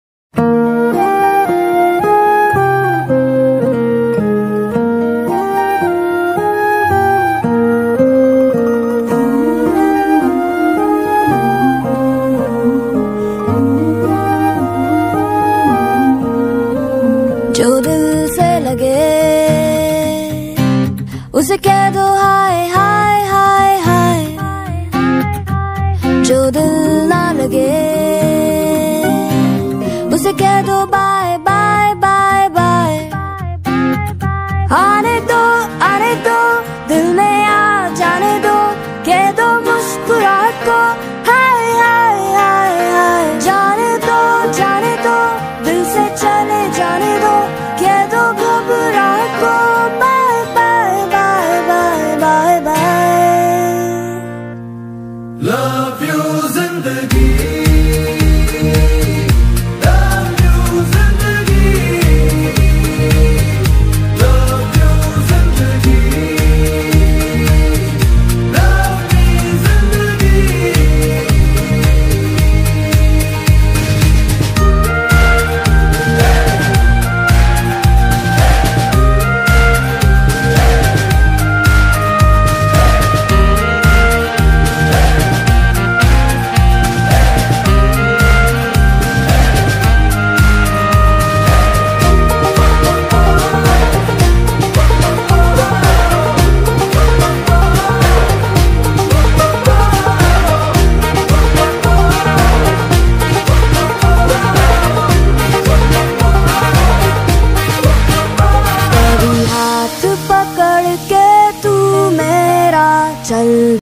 Yoga Acrobat|Yoga Artist|Yoga Flow|Yoga Dance|Yoga Acrobatic
Sonal Yadav and Priyanshi Yadav perfom Acrobatics